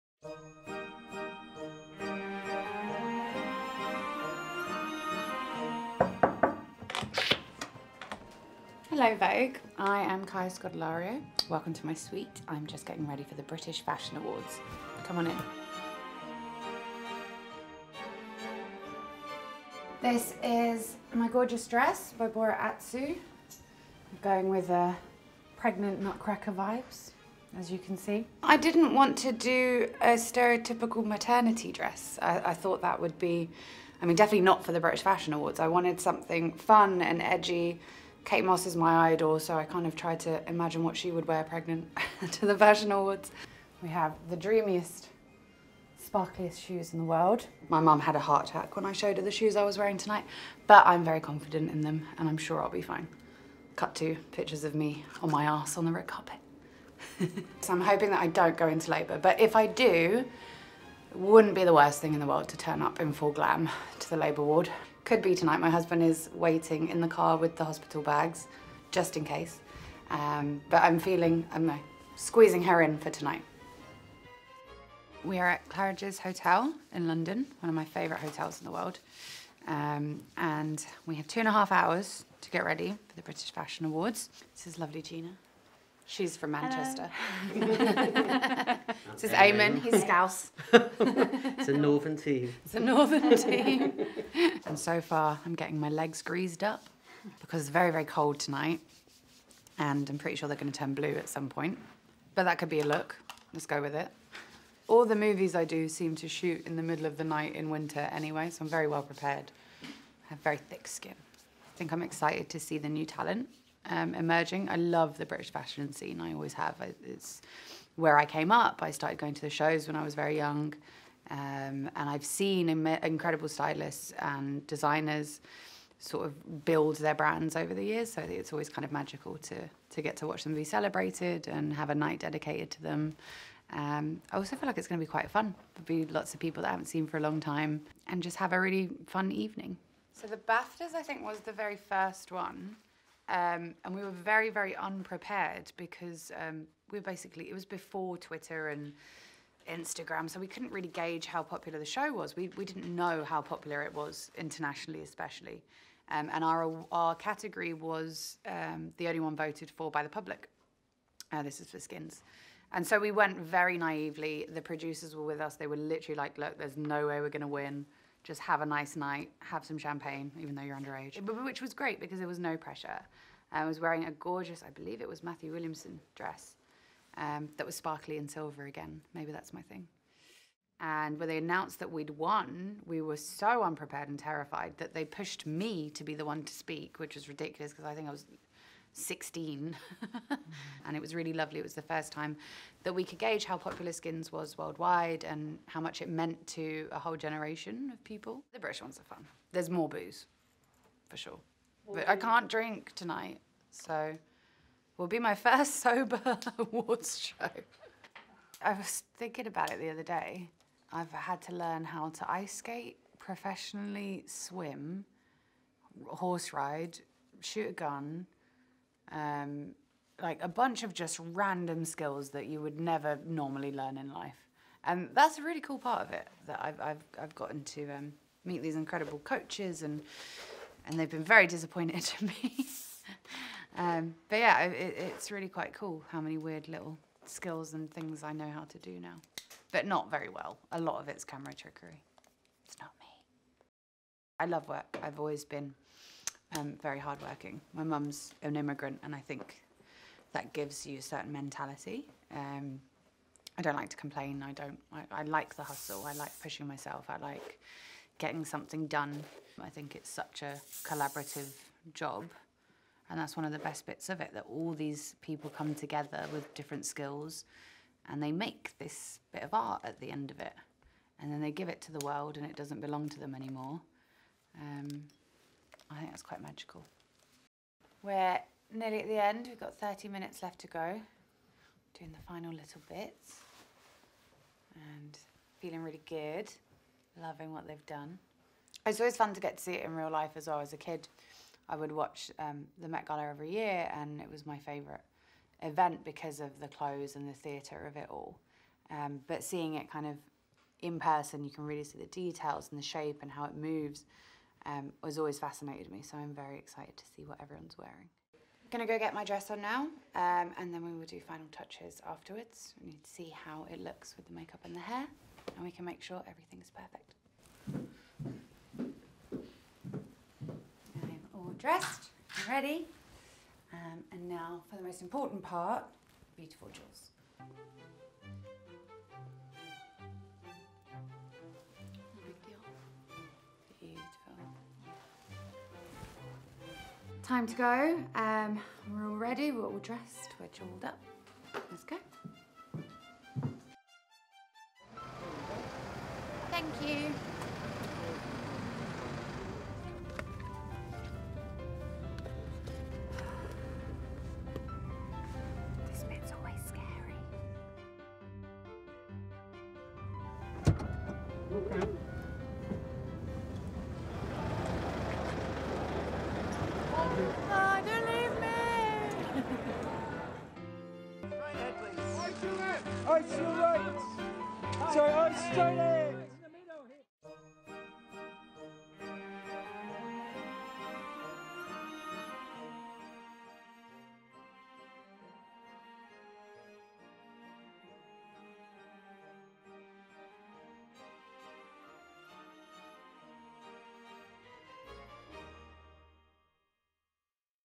Kaya Scodelario Gets Ready for The Fashion Awards at 8 1/2 Months Pregnant | Vogue
Actor Kaya Scodelario was going for “pregnant nutcracker vibes,” while attending the British Fashion Awards.